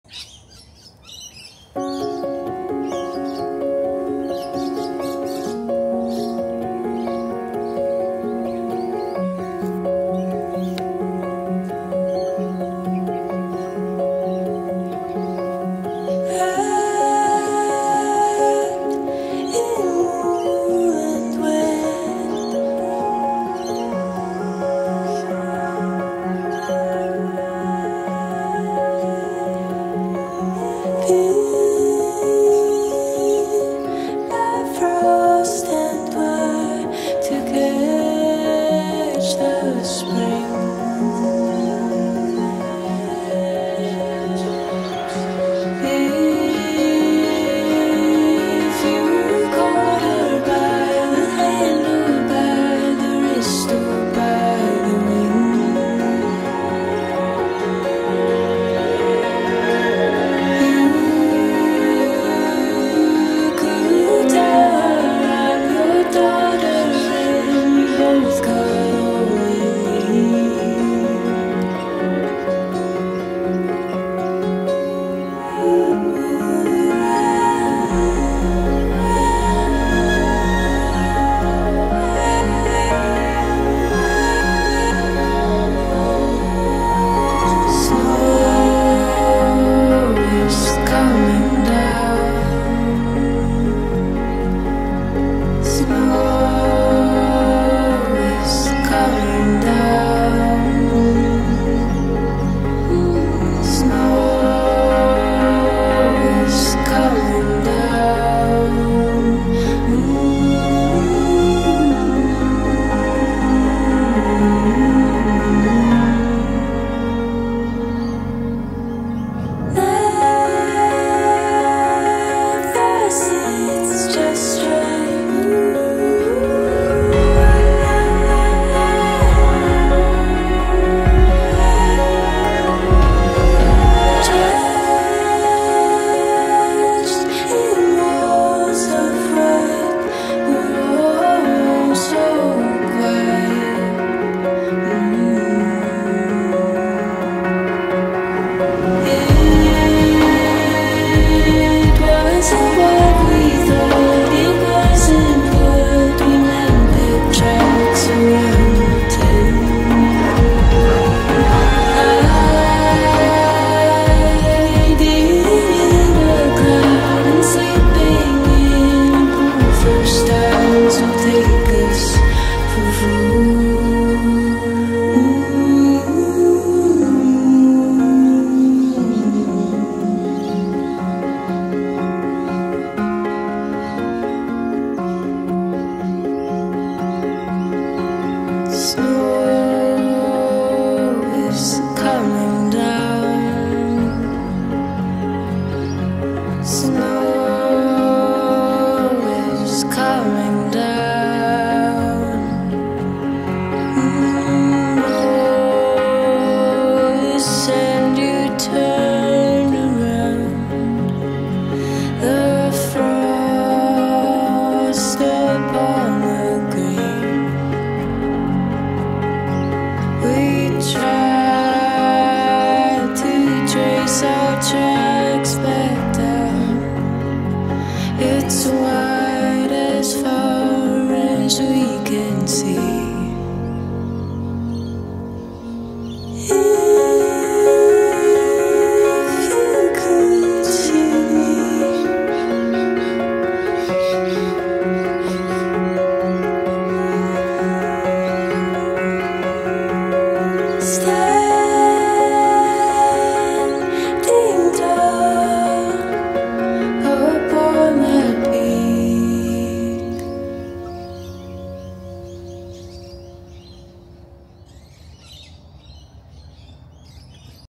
DJI Pocket 3 Straight out of Camera clips, from our garden
We received our recently purchased DJI Pocket 3 just 4 days ago. This is this morning's running around the garden shoot, with totally unmodified footage, straight out of camera. I merely strung together the clips.
© Paul's Media 2024
Music licensed from Audiio.com